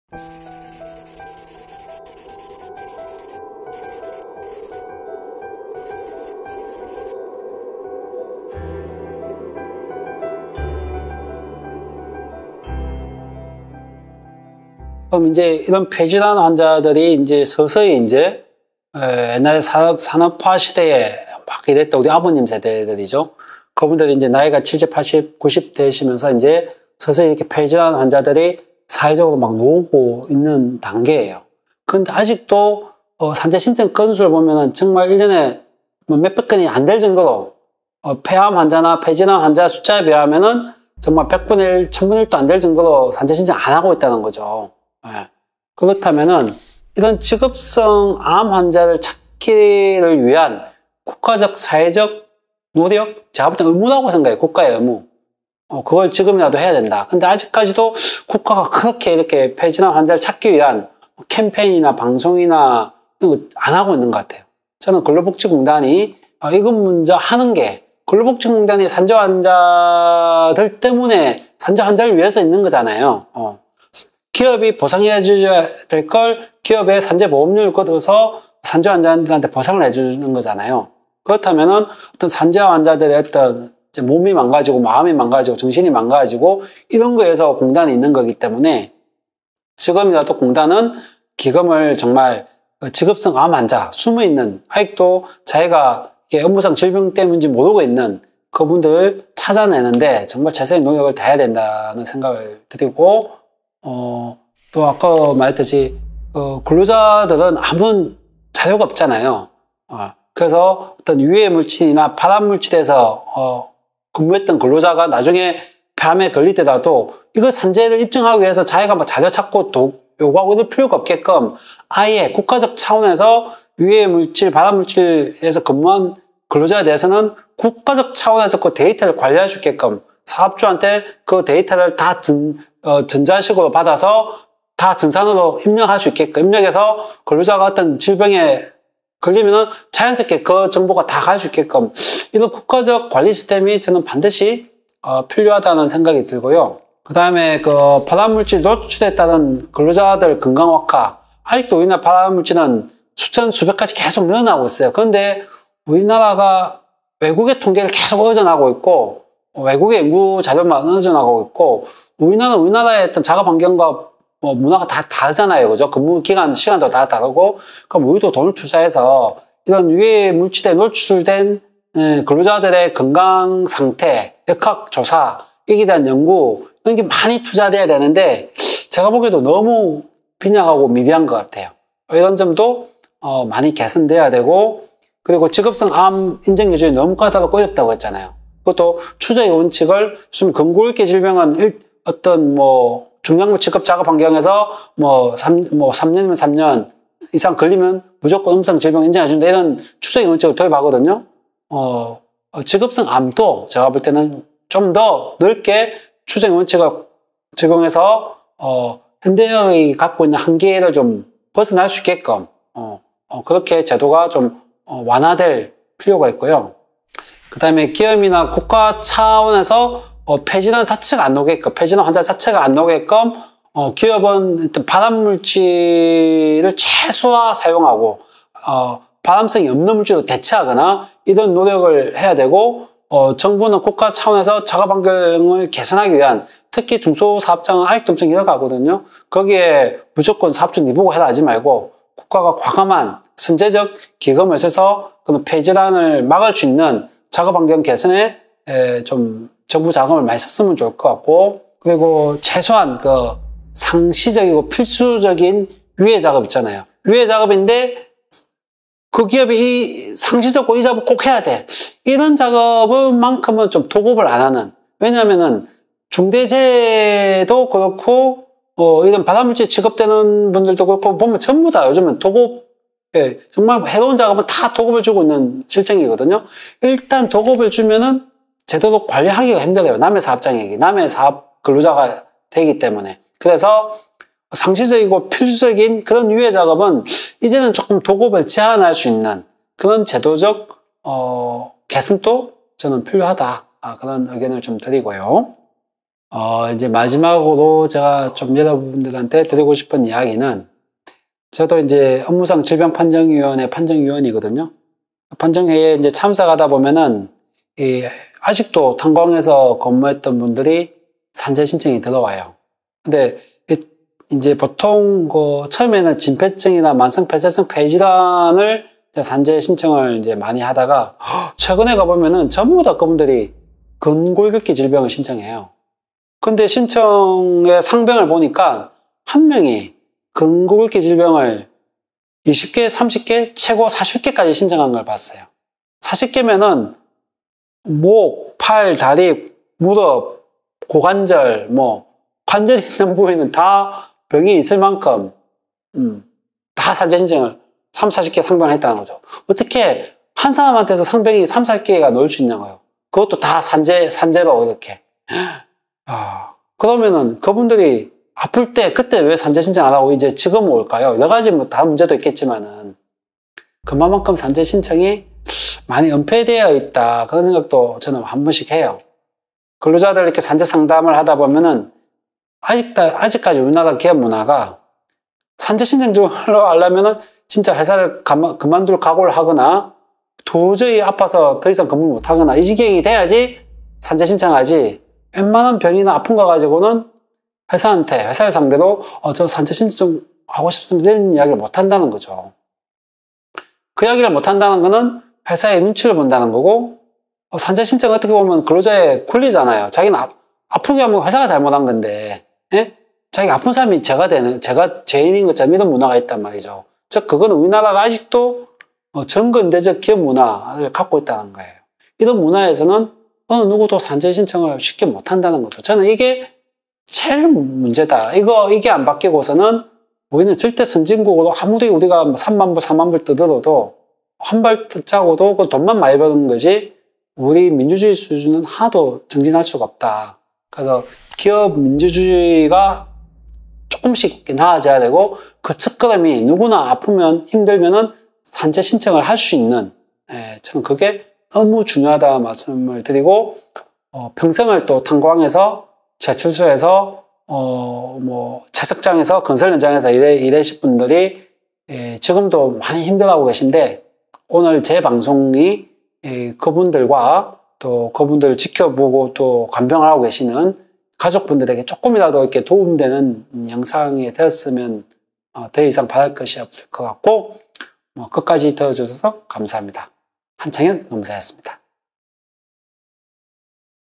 폐질환 예방을 위한 개선방안_보이지 않는 위협, 폐질환 5부 I 사람과산재
[보이지 않는 위협, 폐질환 ] 5부, 폐질환 예방을 위한 개선방안

산재 보상 경력 20년, 노무법인 사람과산재의 대표 한창현 노무사가
그간의 경험을 토대로 업무상 질병으로 가장 많이 발생하는 질환 중 하나인
폐질환을 예방하고 사후대처 하기 위해 나아가야할 방향을 설명합니다.

0:00 인트로
0:46 적극적인 직업성 암 환자 탐색
1:44 유해물질 노출 이력 관리시스템
2:27 유해물질의 영향에 대한 연구확대
3:05 직업성 암 산재 인정기준의 완화
3:37 기업/국가 차원의 작업환경 개선
4:12 상시/필수 유해작업 도급 제한
4:56 산재 신청이 자유로운 기업 문화

사람과 산재 대표번호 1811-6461